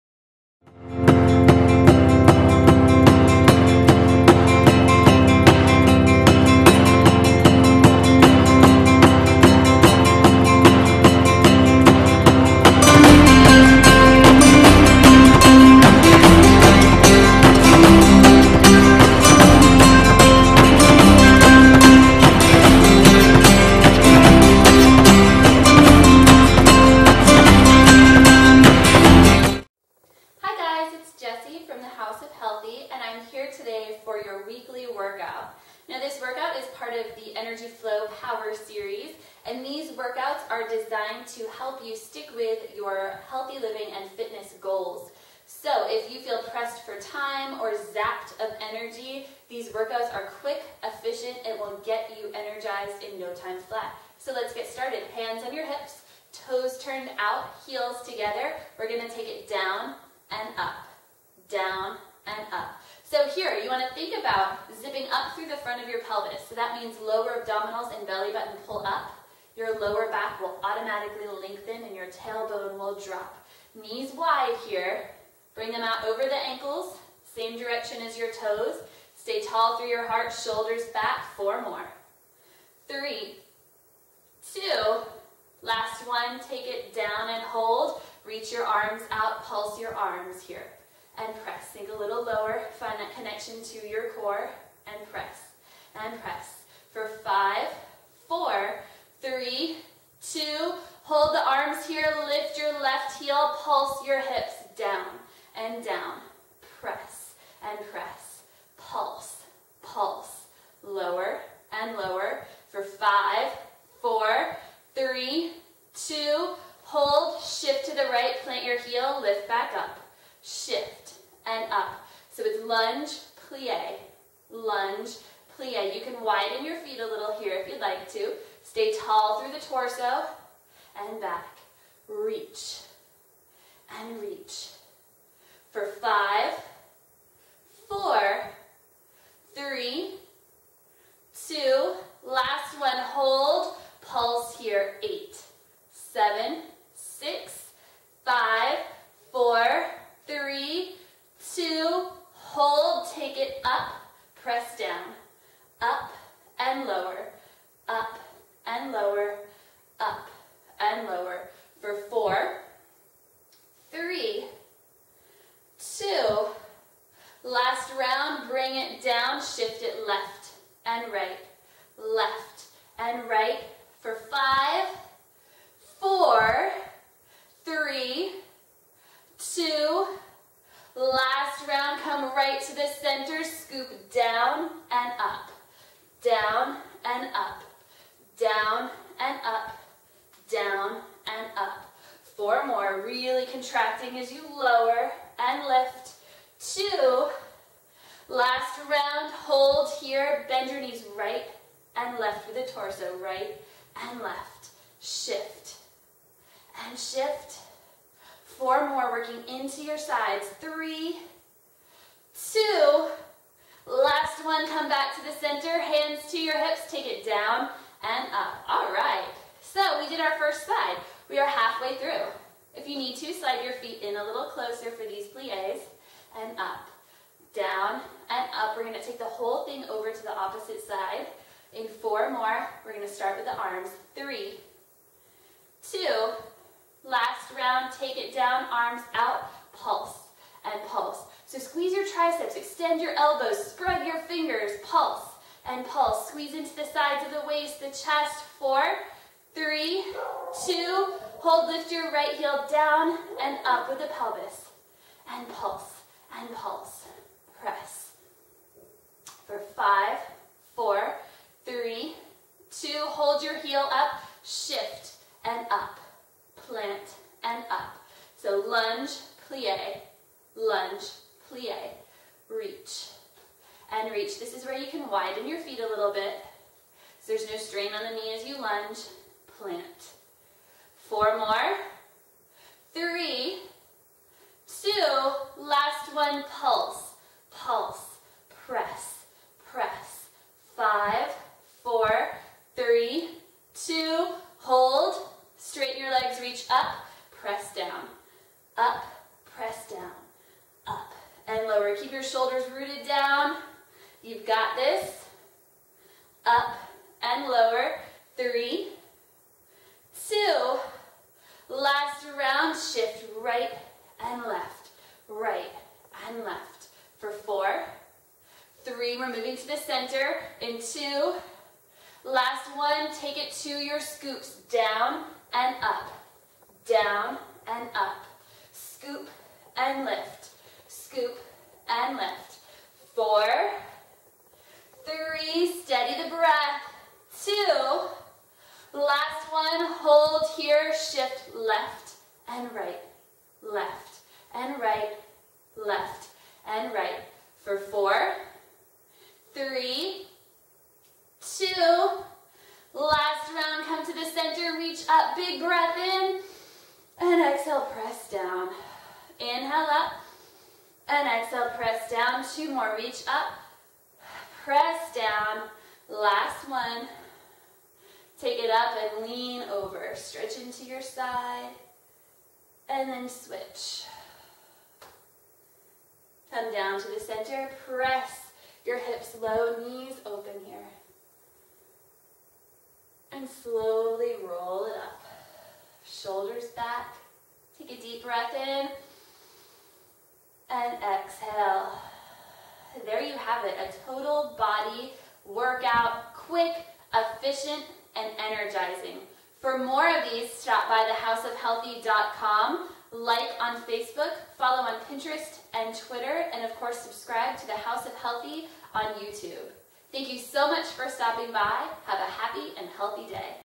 The House of Healthy: Energy Flow Power Series Week 2
Welcome to the House of Healthy Weekly Workout! This week is a 3-part series called Energy Flow Power Moves. These are quick,10 minute versions of my signature workout and class, Energy Flow. These are powerful, efficient workouts designed to give you an energized, total body glow.